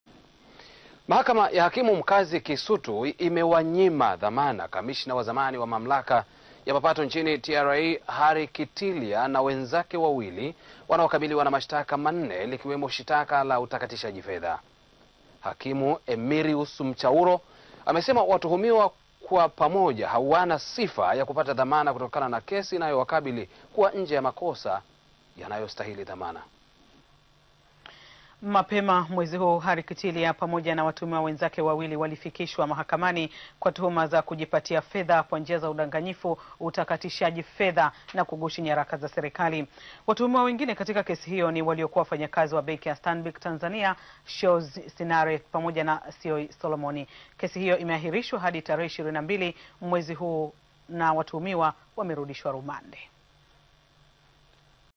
Mahakama Kisutu Yawanyima Dhamana Watuhumiwa Utakatishaji Fedha
Mahakama ya Kisutu ya jijini Dar es salaam yawanyima dhamana kamishina mkuu wa TRA wa zamani pamoja na wenzake 2 ambao kwa pamoja wanakabiliwa na mshitaka 4 ikiwemo utakatishaji fedha.